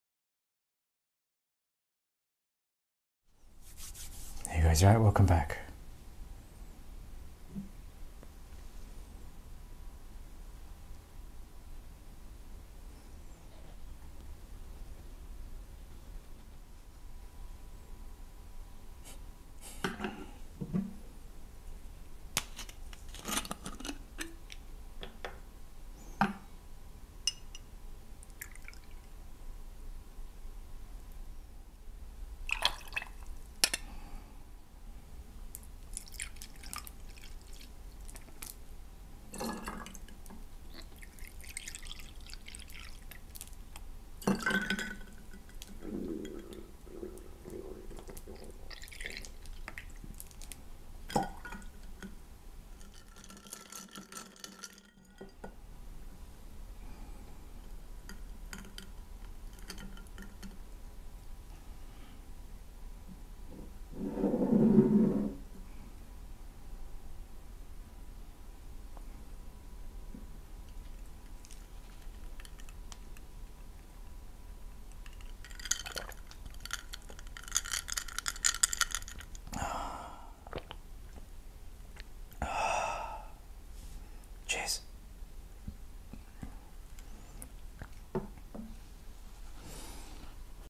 A Bitter Beauty! GARIBALDI Cocktail.
Sweet and bitter is a winning balance.

INGREDIENTS
11/2oz /45ml Campari
4oz/ 120ml Orange juice
Orange wedge or peel